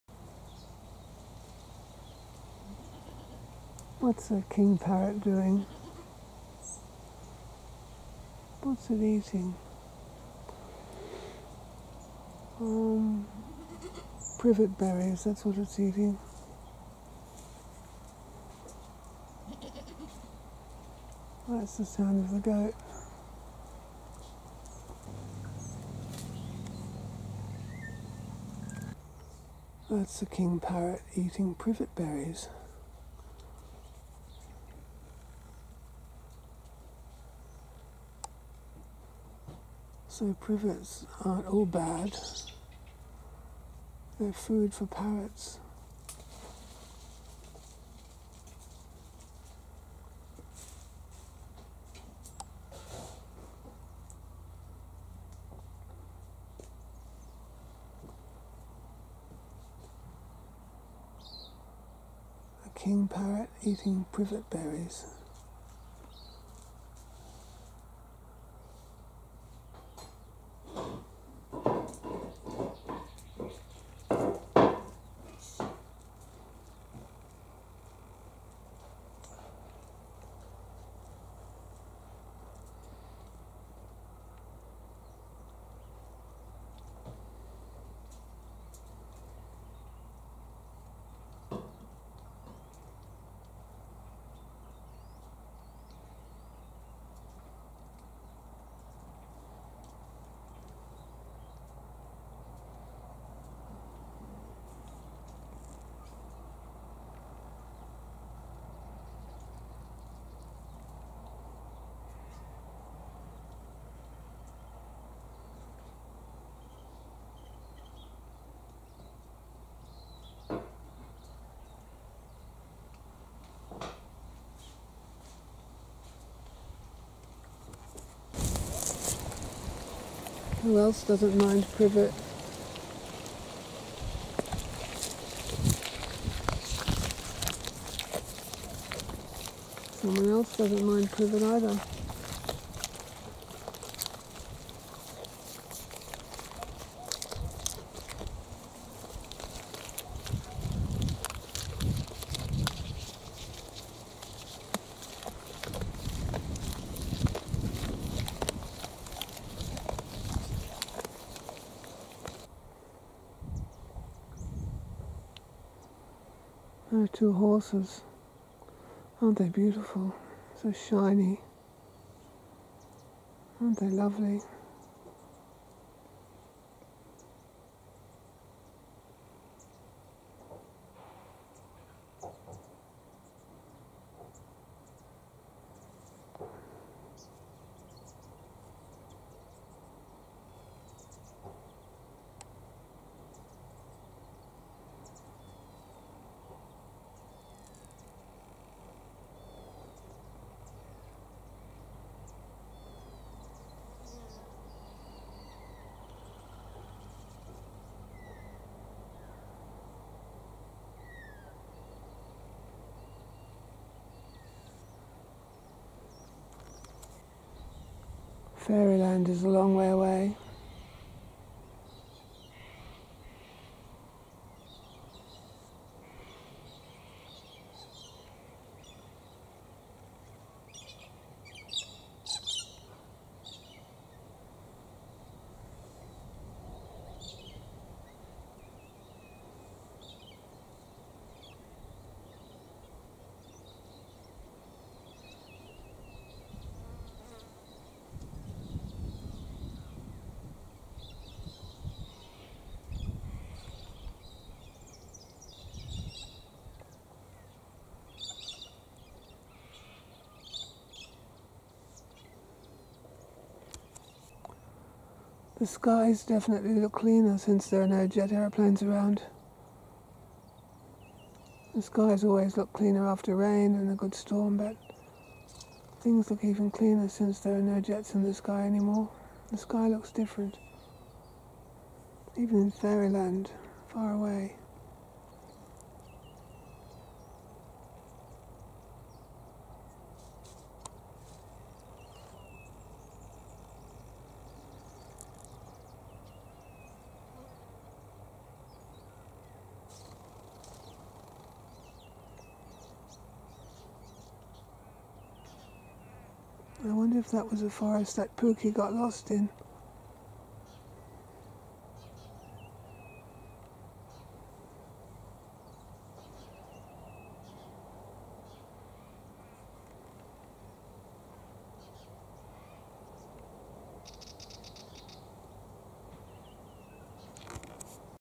Privets, Parrots, Goat, Horses and More... 🌿🦜️🐐 🐎 🐴 🧚‍♂️
Privet berries are eaten by parrots. Privet leaves are enjoyed by goats. So that shrub isn't all bad. A look at two horses, and the distant fairyland...